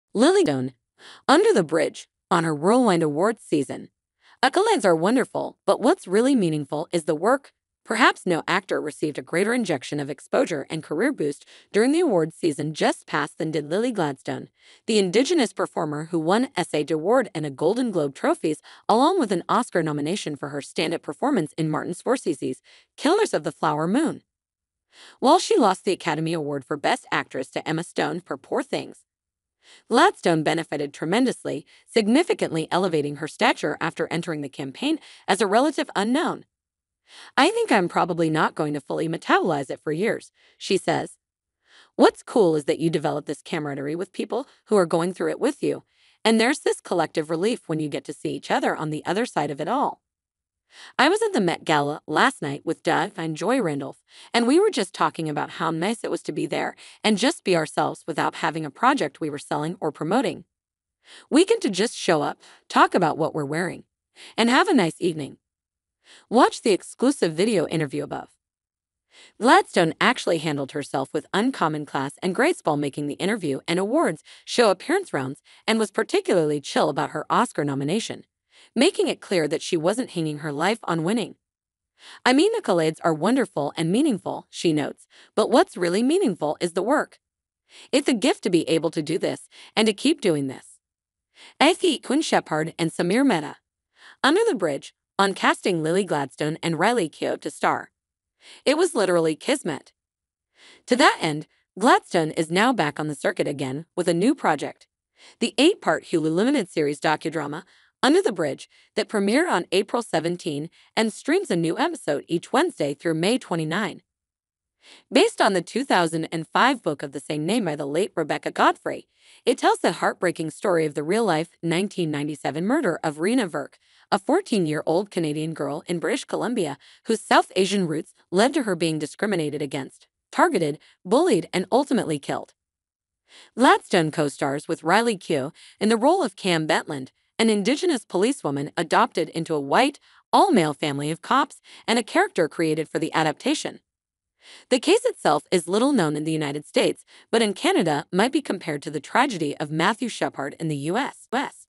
Lily Gladstone (‘Under the Bridge’) on her whirlwind awards season: ‘Accolades are wonderful,
Lily Gladstone, an Indigenous performer who won SAG, Golden Globe, and Oscar awards, has praised her career boost during the awards season. Despite losing the Academy Award for Best Actress to Emma Stone, Gladstone found solace in the camaraderie she developed with others. She is now working on an eight-part Hulu limited series docudrama, "Under the Bridge," based on the 1997 murder of Reena Virk.